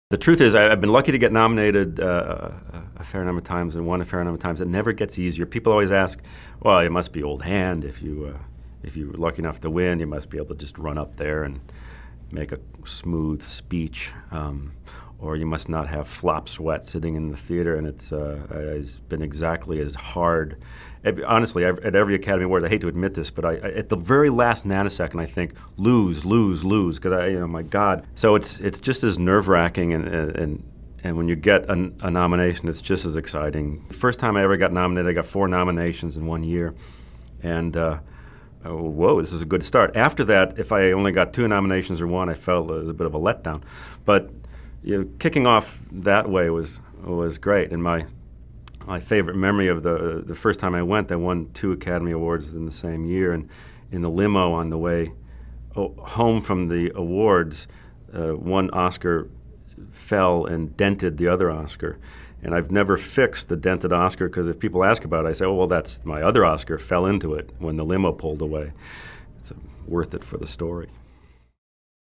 Gary Rydstrom on winning two Oscars® in one year
Sound designer/editor Gary Rydstrom has been nominated for an Oscar® fourteen times and won seven but it's still nerve-racking.